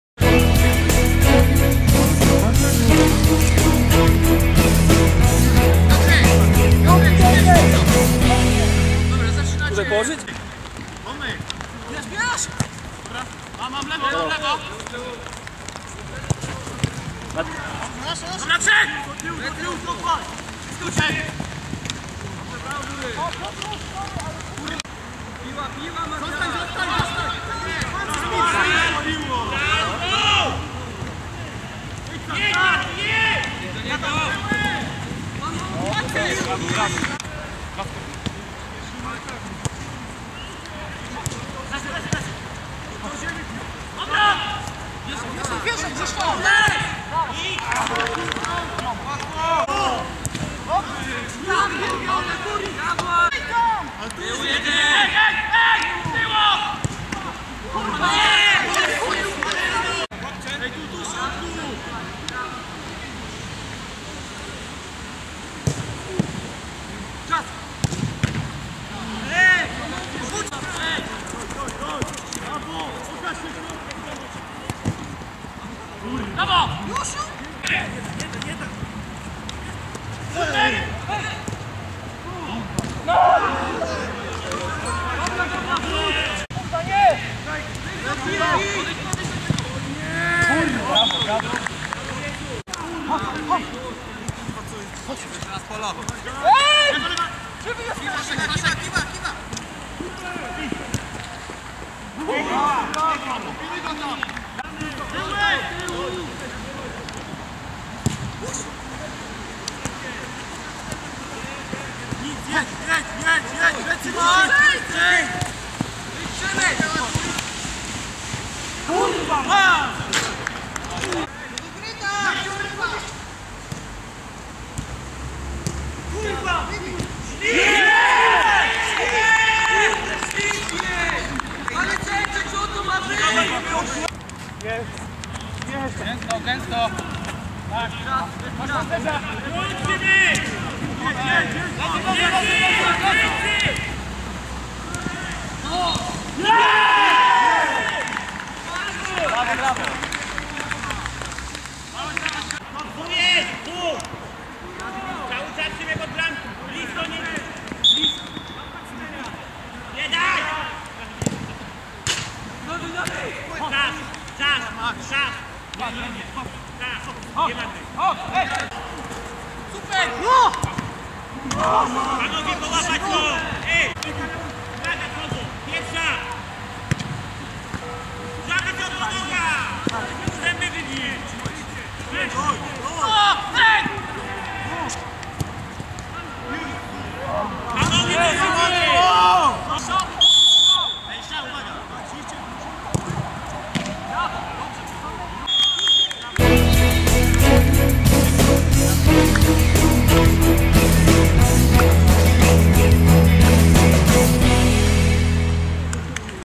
11. tydzień: Dzida w Przód - DTW-Galacticos (FLS Wiosna 2013)
Skrót meczu Dzida w Przód - DTW-Galacticos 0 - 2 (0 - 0)